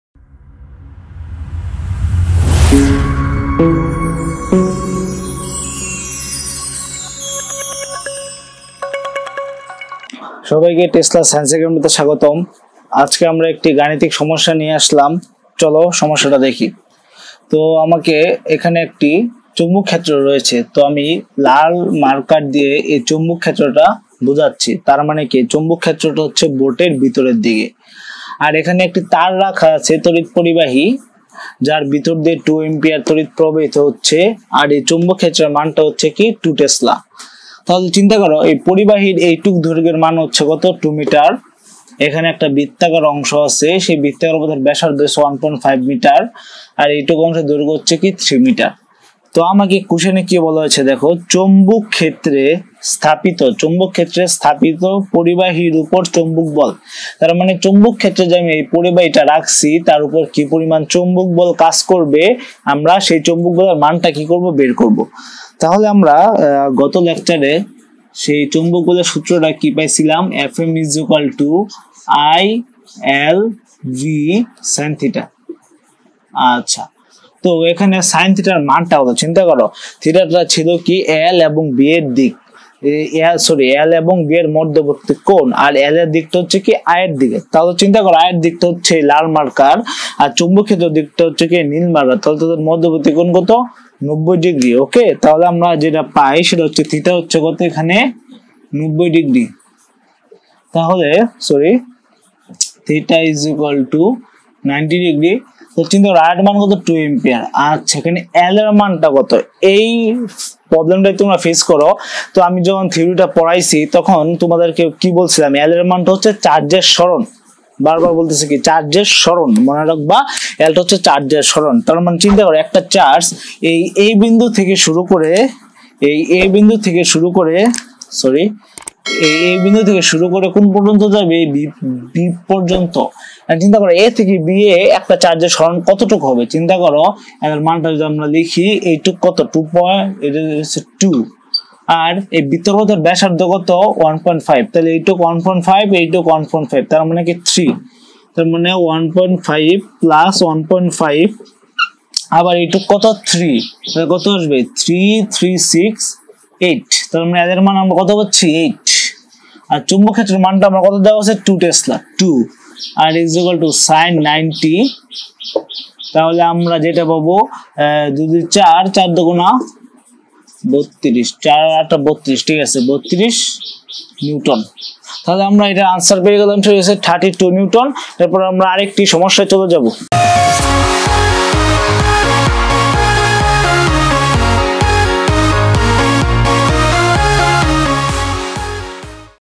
গাণিতিক সমস্যা-০১(চুম্বক ক্ষেত্রে তড়িৎবাহী পরিবাহীর ওপর চুম্বক বল)
HSC PHYSICS 2ND PAPER 4th CHAPTER
CHAPTER NAME : Magnetic effects of electric current and magnetism
LECTURED BY : MD.KAWSHAR AHMED POROSH
Mechanical Engineering,Batch-20­18,IUT